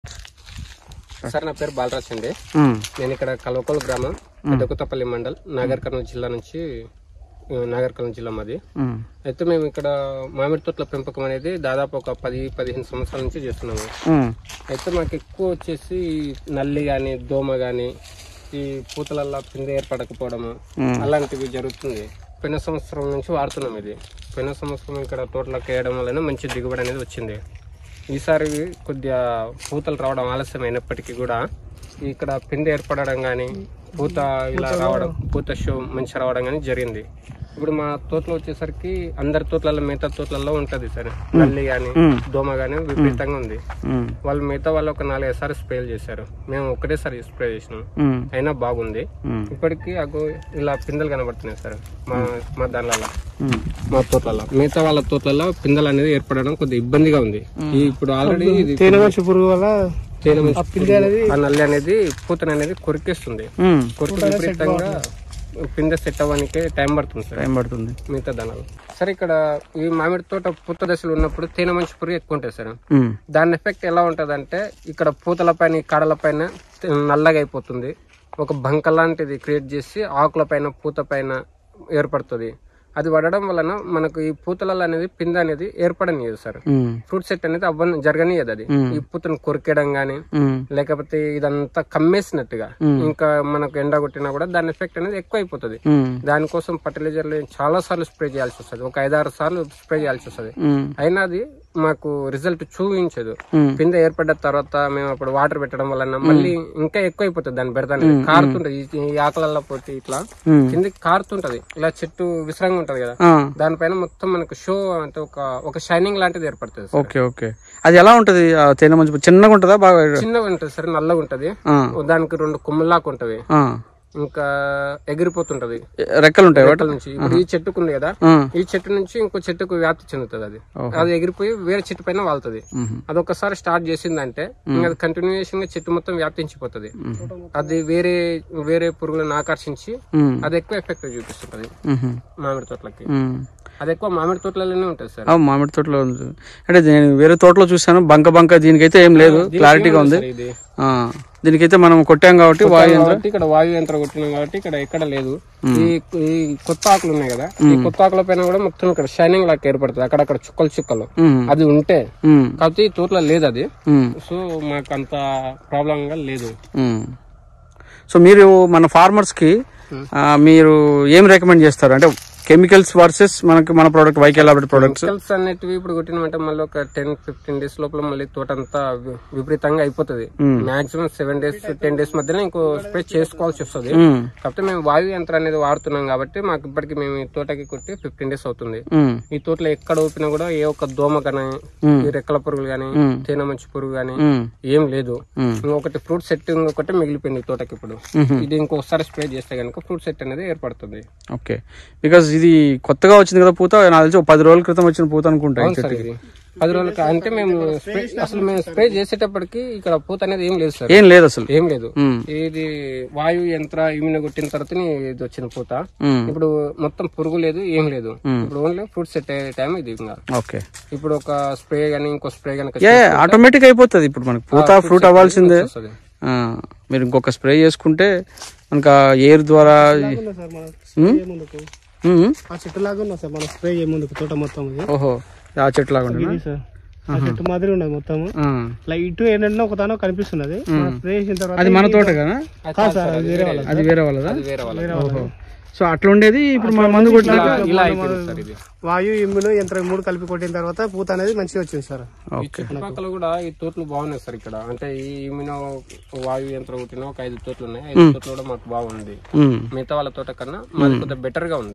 లో తేనె మంచు పురుగుHoney plant hopper# నల్ల తామర నివారణ#Vaayu Yantra
YK Laboratories Mango Special for Flowers & Fruit Setting
Click here For Amazon Orders :

Dosage :-Immuno 3 ml ,Vaayu 3 ml ,Yantra 3 ml per Ltr (For only mango )
Repeat after 15 days

Mango Plant Hopper#
Mango Insects#
Mango Blackthrips.
మామిడి లో తేనె మంచు పురుగు
Mango Flowers
Mango Fruit setting